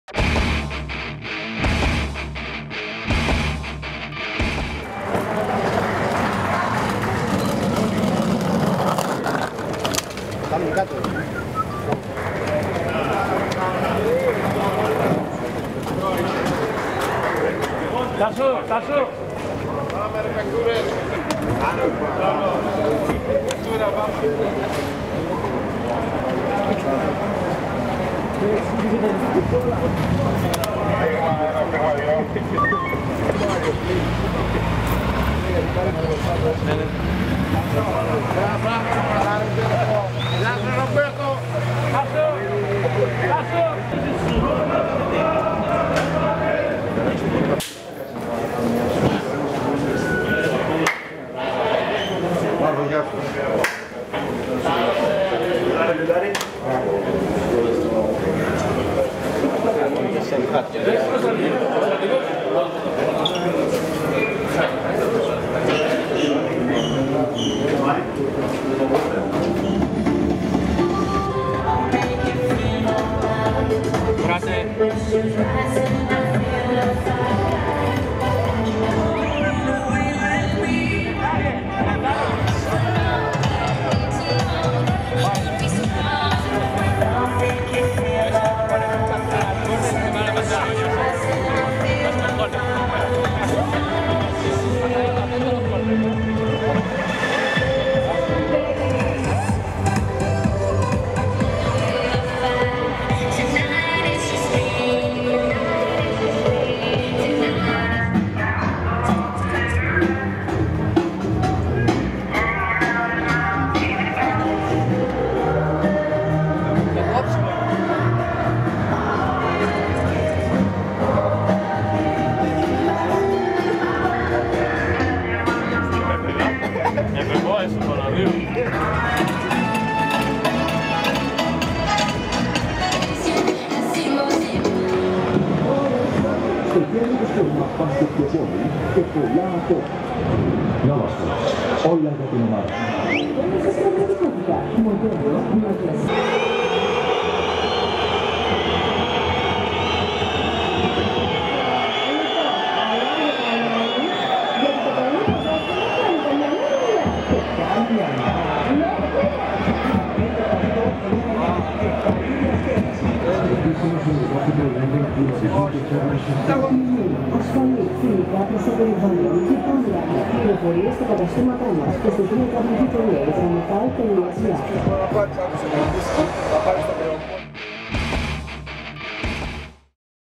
Pregame του ΟΦΗ - Ολυμπιακός / Pregame of OFI - Olympiacos
Δείτε όλα όσα έγιναν πριν από τον αγώνα ΟΦΗ - Ολυμπιακός για το Κύπελλο Ελλάδος.